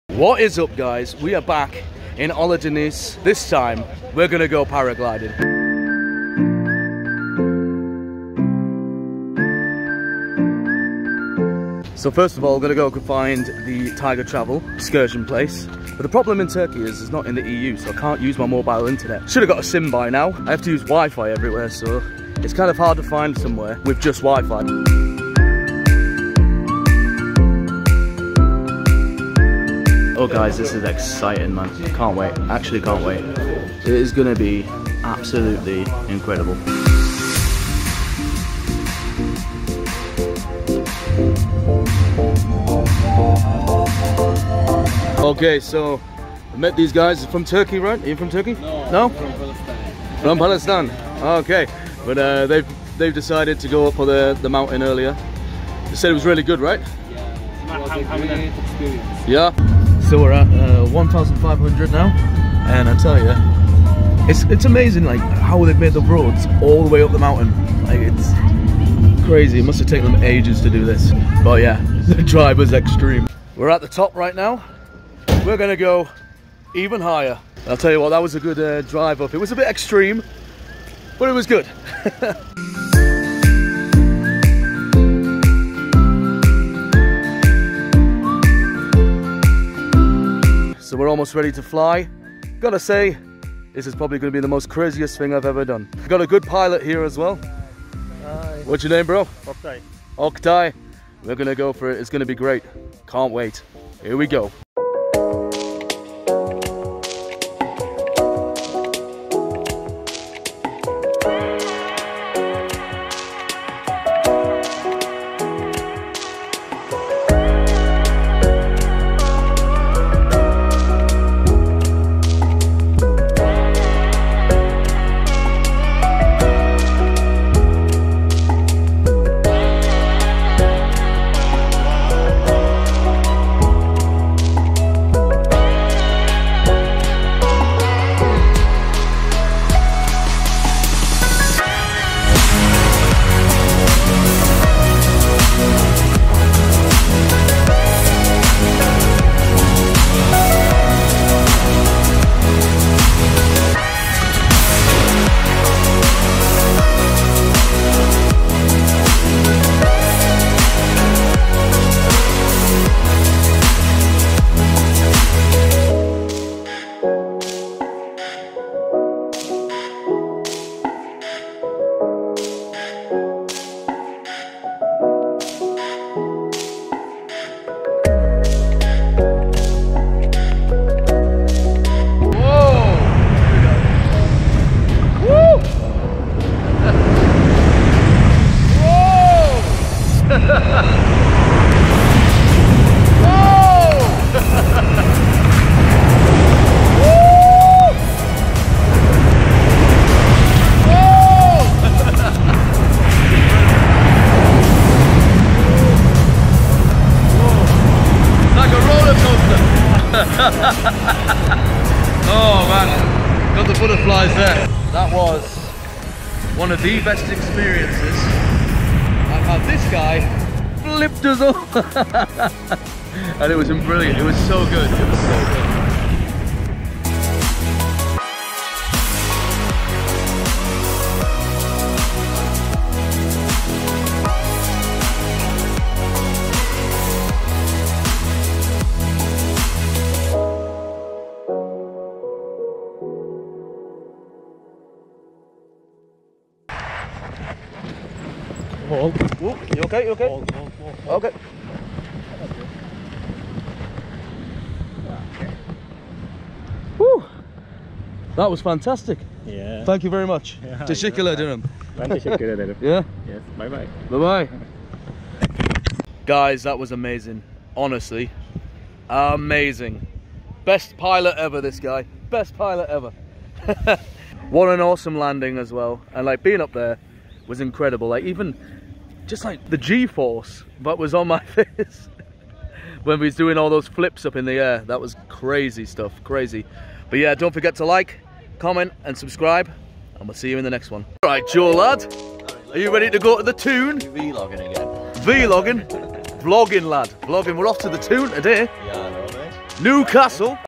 🇹🇷 I ALMOST DIED PARAGLIDING IN TURKEY
SO I DECIDED TO RETURN TO BABADAG MOUNTAIN IN OLUDINEZ TO PARAGLIDE OF THE WORLDS MOST INSTAGRAMMABLE MOUNTAIN!

THE EXPERIENCE WAS LIKE NO OTHER IT WAS AMAZING!